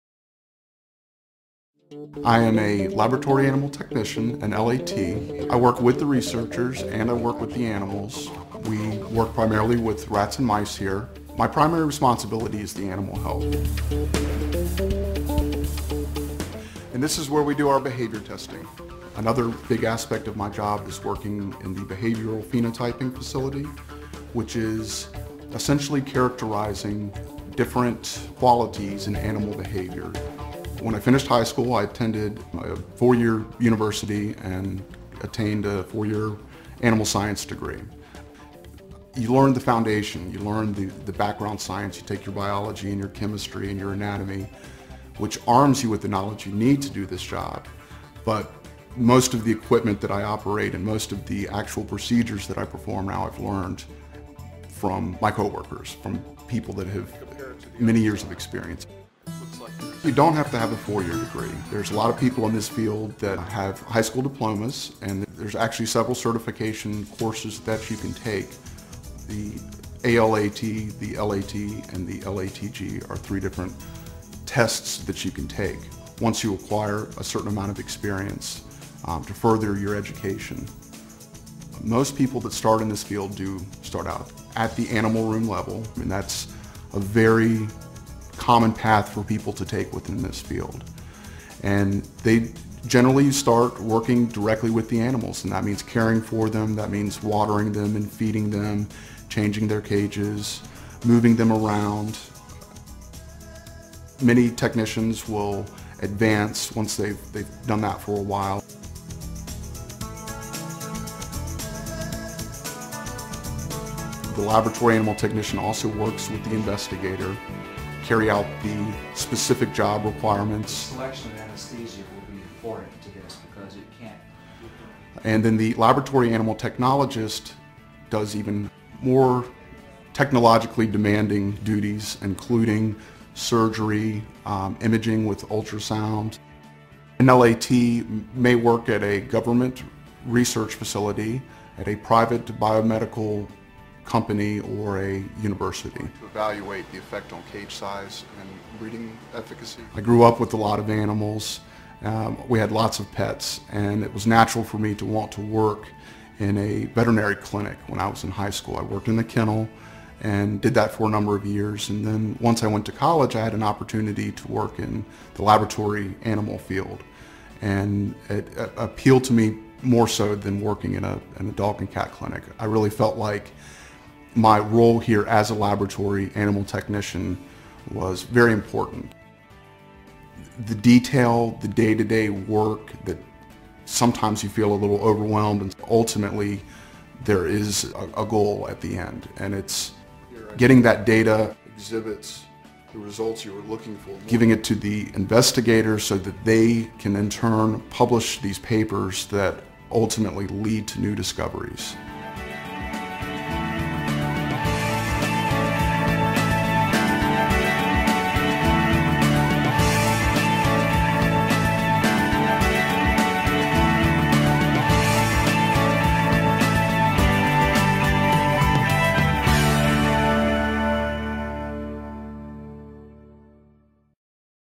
Laboratory Animal Technician
Learn all about the career of a laboratory animal technician -- including how much money they typically make and how much education you need to become one.